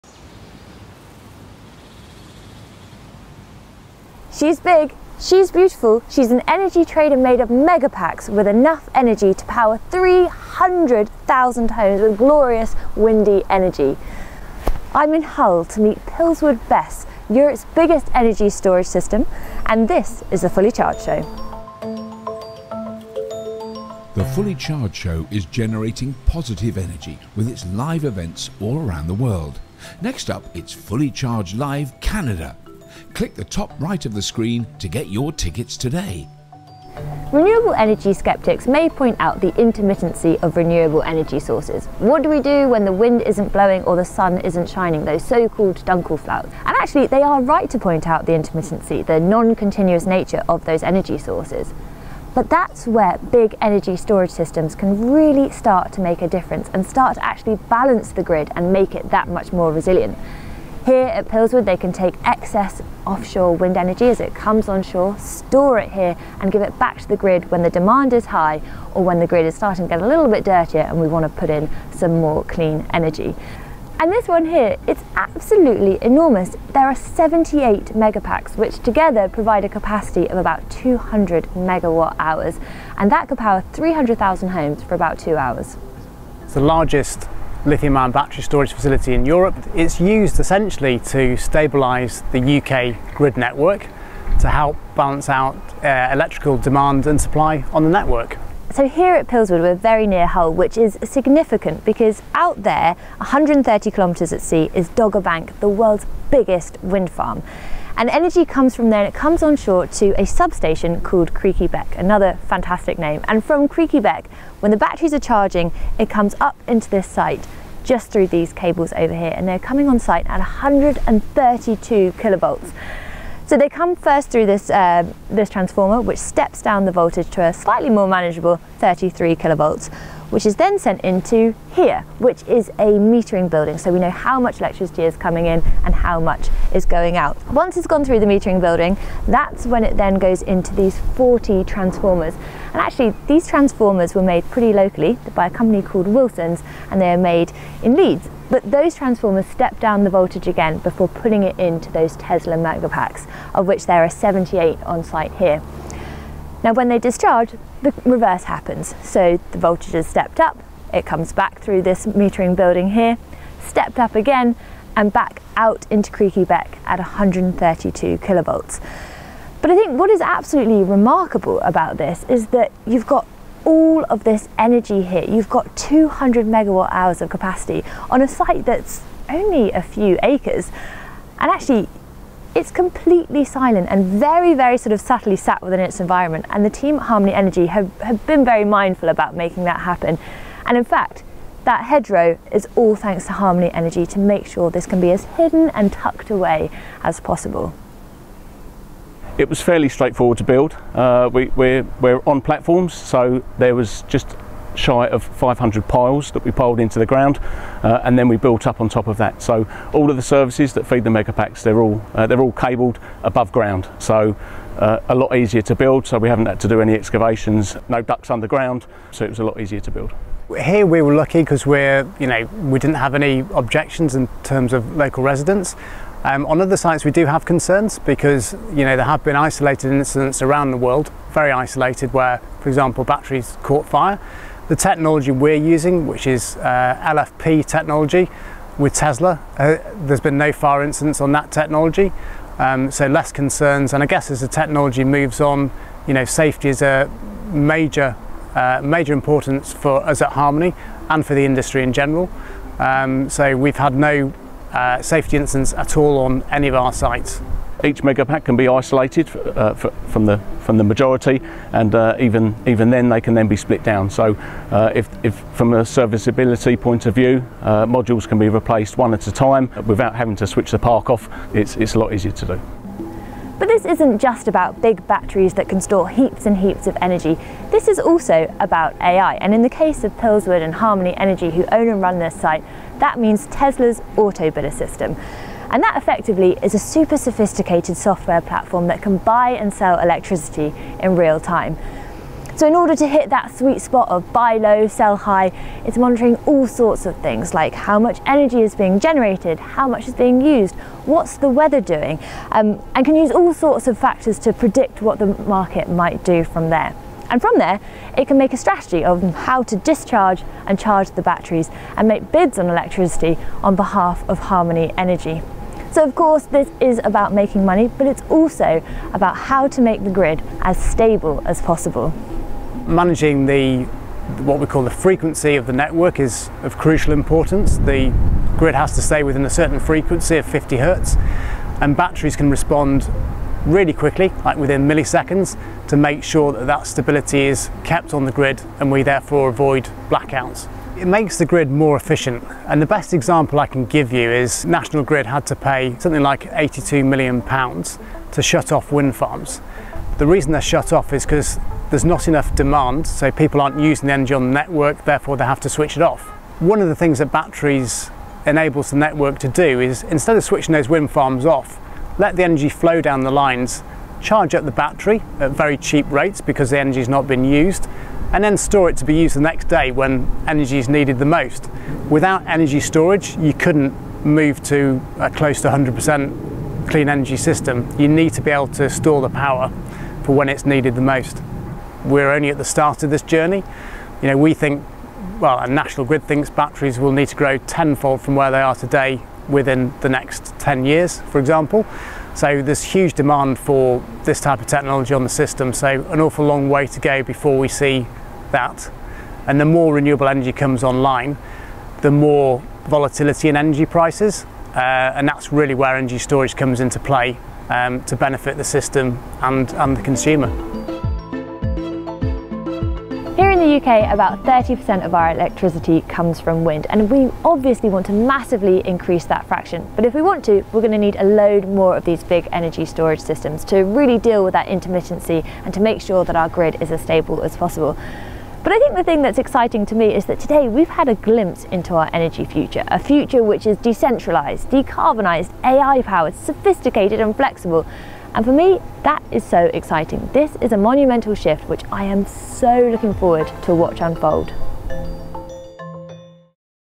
MASSIVE Storage. THIS is How To Power the Grid With 100% Renewable Energy!
Big batteries are perhaps the key to making a completely renewably powered grid possible. Luckily there are already some massive ones paving the way. This week Imogen visited Europe's largest battery energy storage system, Pillswood in Hull to find out how Harmony Energy is getting as much clean energy from Dogger Bank, the world's biggest wind farm, onshore and into our homes. But these are no ordinary batteries, at 200 MWh these Tesla Megapacks are decked out with Artificial Intelligence, these are working to make the grid both renewable and flexible.

00:00 Massive Megapacks
00:36 Intermittency of Renewable Energy
01:28 Largest Storage in Europe
01:53 Charging and Discharging
03:19 How do you build it?!
04:33 Artificial Intelligence
05:26 Managing the Grid
06:12 100% Clean Energy System
06:52 The Future of Energy?!

For Clean Energy and Home Tech take a look at the @EverythingElectricShow